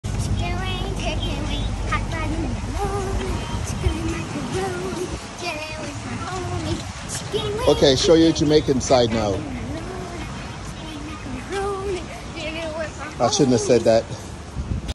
This is NOT the Jamaican Dance I Was Expecting
This is NOT the Jamaican Dance I Was Expecting from him. He is not acting like a true Jamaican dancing like this.